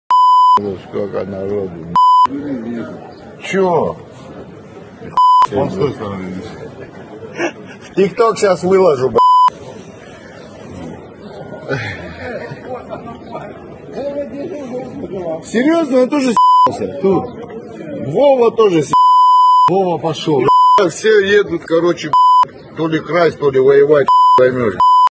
РФ вербує ув’язнених на війну в Україну
Телеканал Донбас 24. Найбільший регіональний телеканал.